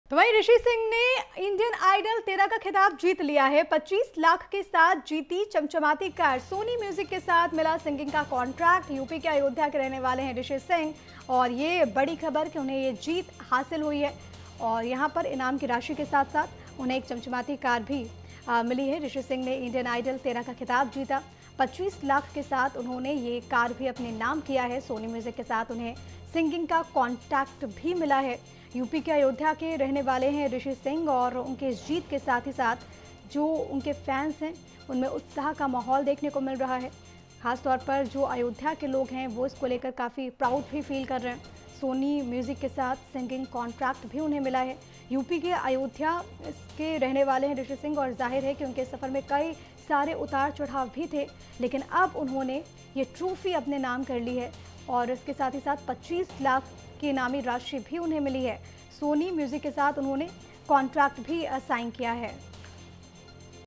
Indian Idol 13 Winner: Ayodhya के ऋषि सिंह ने चमचमाती ट्राफी के साथ जीते 25 लाख रुपये, देखें रिपोर्ट
Social Media -